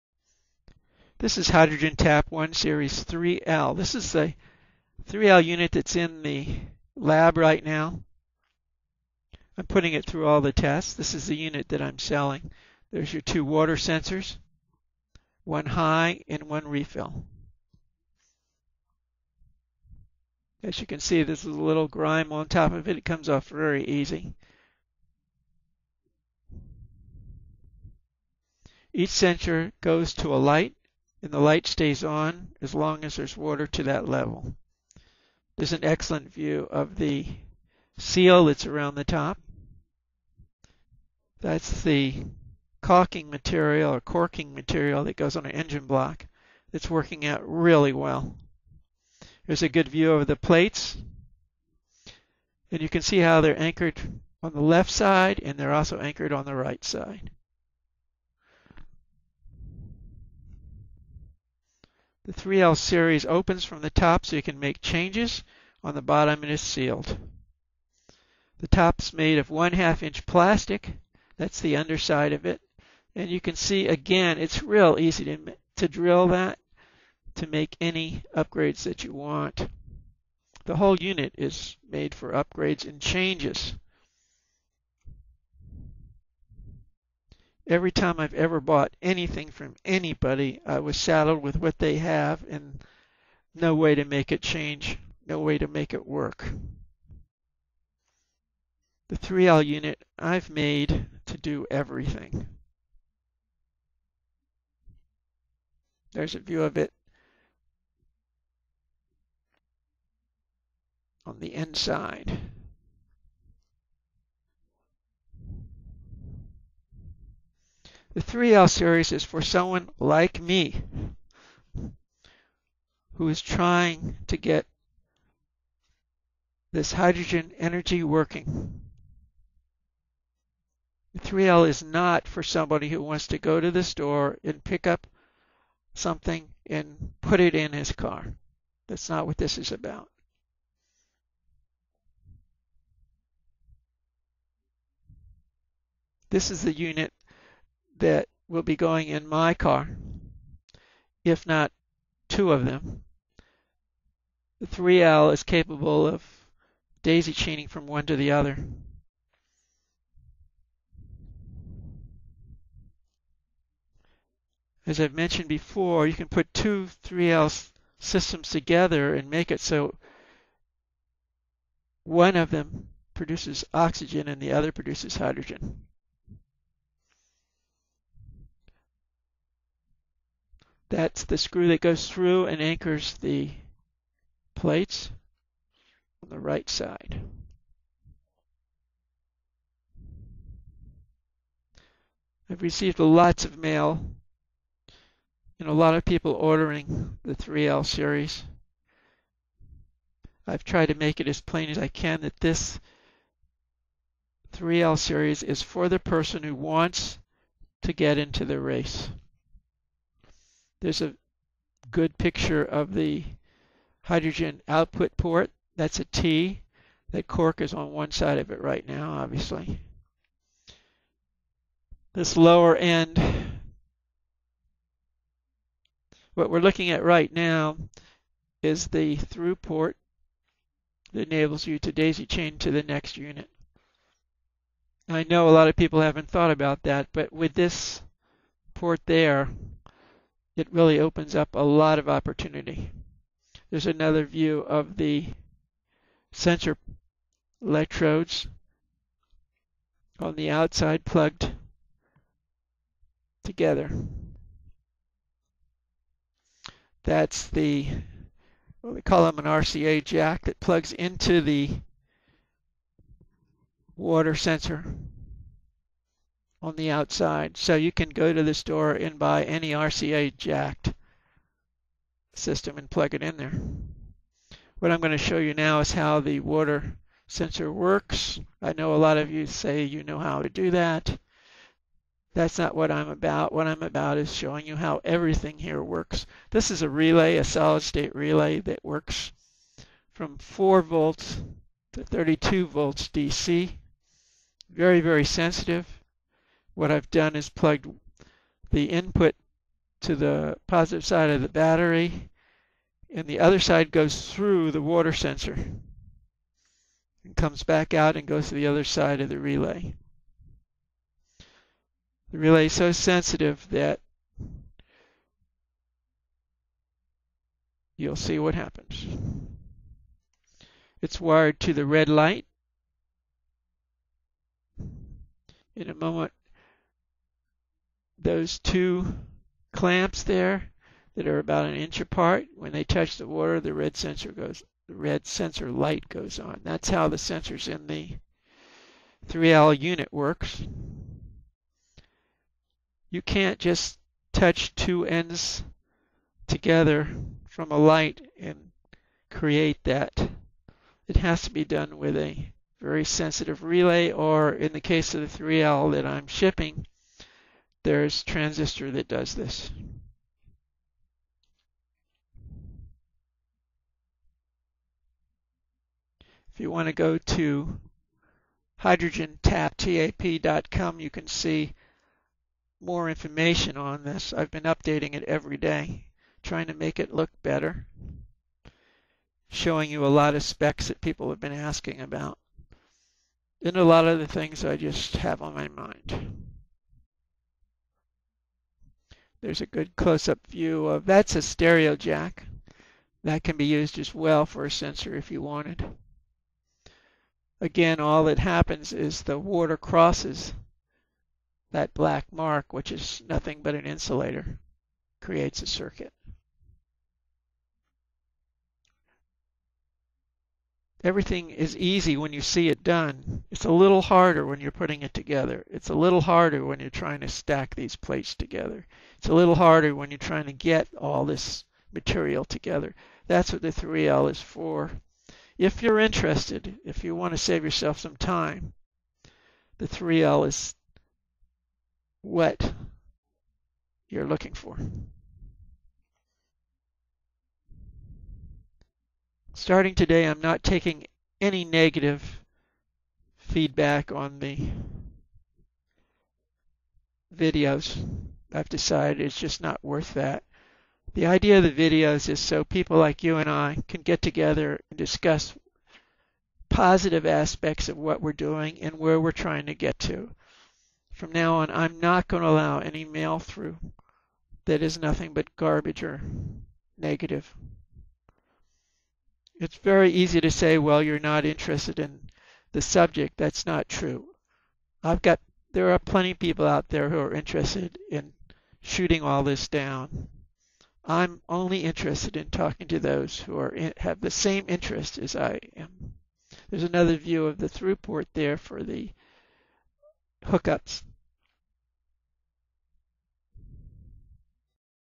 Hydrogen Tap 1 Series 3L (More Views after Test) #51
This video shows the 3L unit after 2hour test to see if new seal and gold plated sensors would hold up during run. Also views of the way the plates are attached to the unit on the inside. There great views of the inside of the unit and a short demo of how the water sensors work. This video shows more sides of he 3L unit that were not included in the last. For more information on the 3L check "hydrogentap.com" I have added more data that you might be interested in.